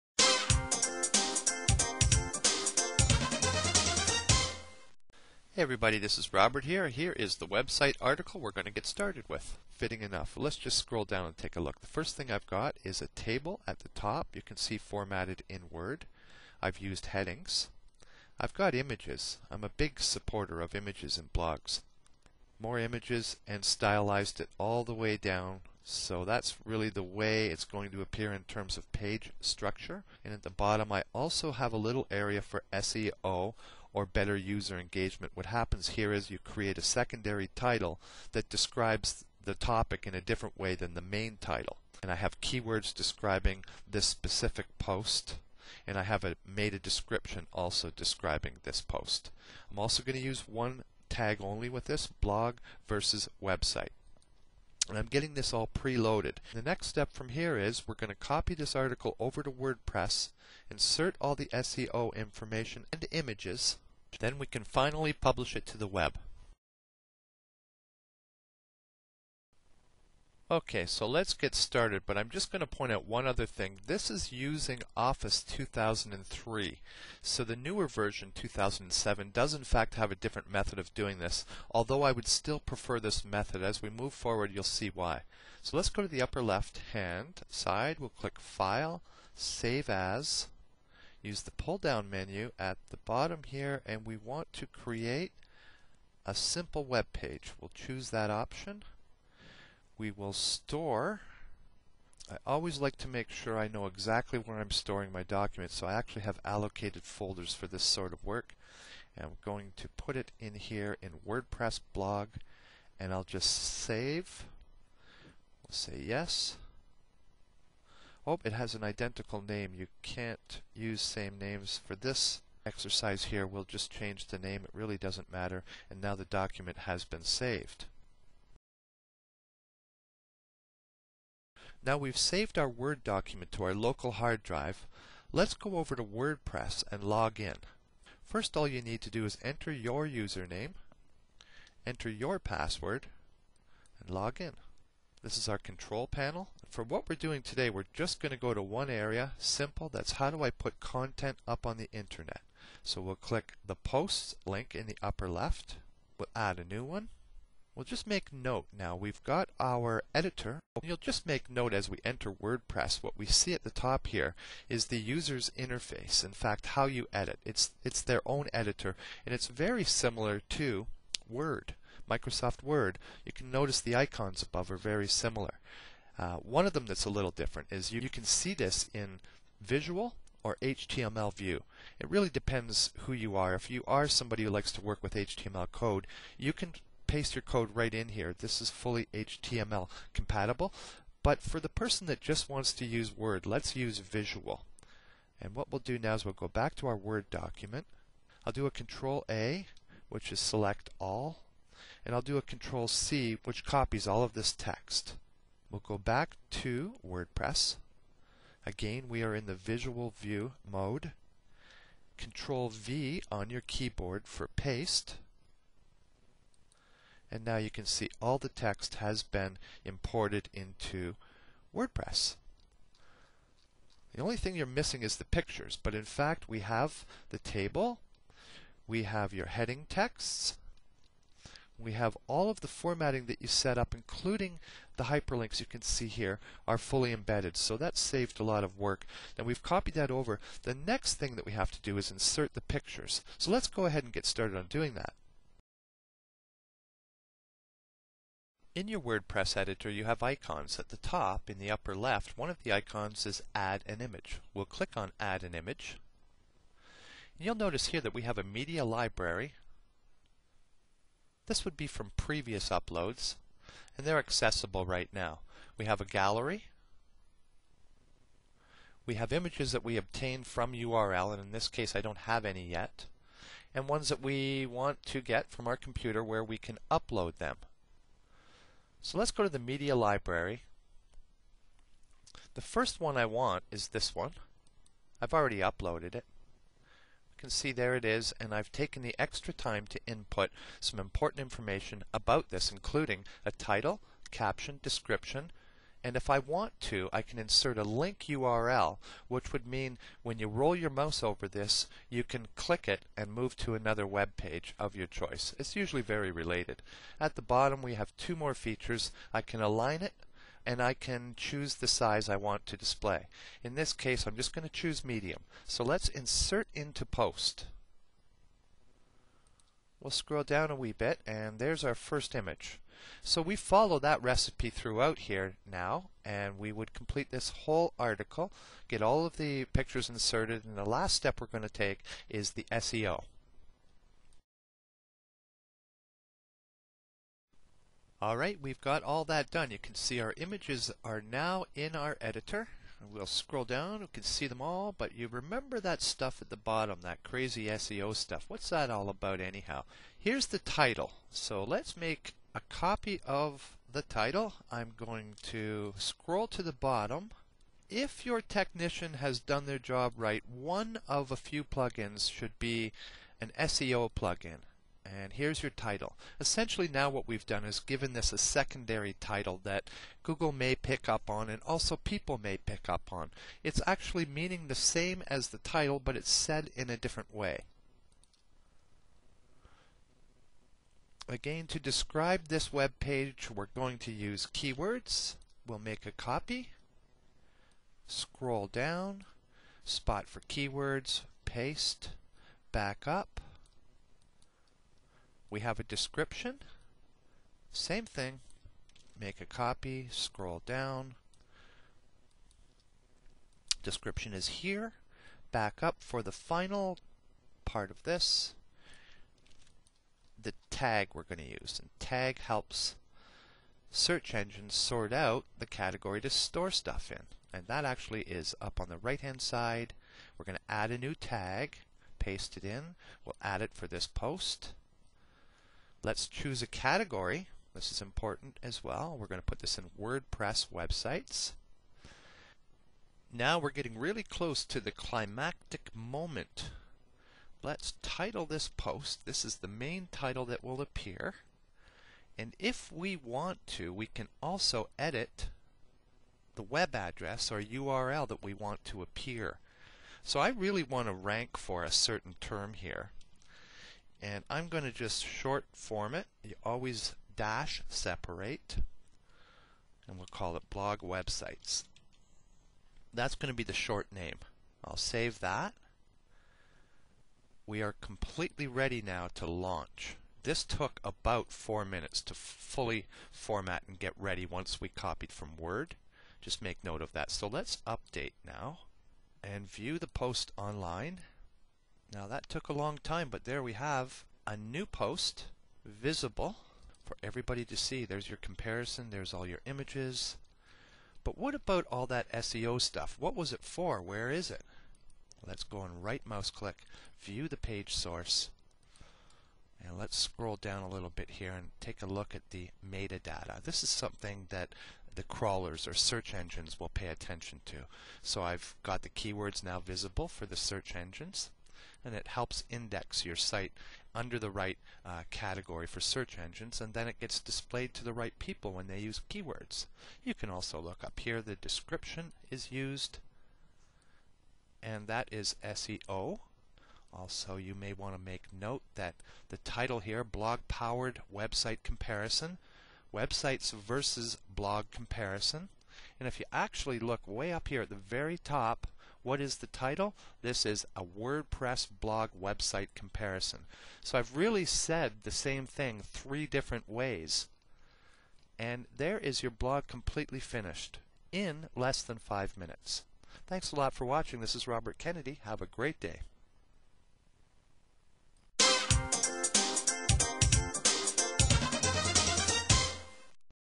Creating Wordpress Blog Posts From MS Word
Not everybody has the techical savvy needed to create webpages. Most people in business do know how to use Microsoft Word. What would you say if I told you, that's all you need to know! Creating your blog posts in Wordpress is so easy it may cause you to think about getting WordPress to power your website. That's not a crazy thought, it's content management!